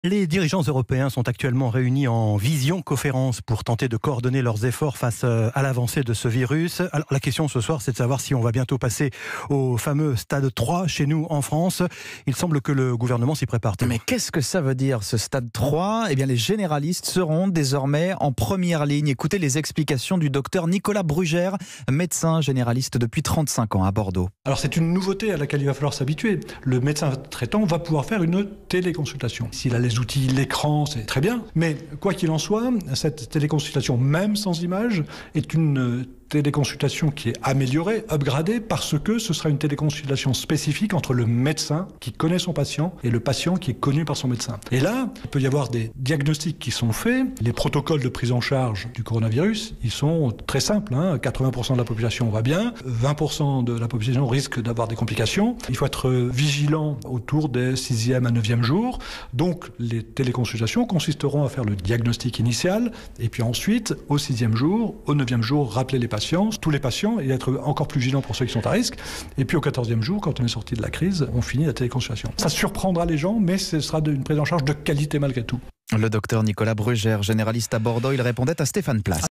Coronavirus : ce que change le "stade 3" de l'épidémie pour les médecins généralistes
Alors que la France a enregistré sa plus forte augmentation de cas de coronavirus sur 24 heures, mardi, le gouvernement semble se préparer au passage au stade 3 de l'épidémie. le docteur Nicolas Brugère expliquait sur Europe 1 que, pour les médecins généralistes, il serait désormais possible de mener des téléconsultations à distance avec leurs patients.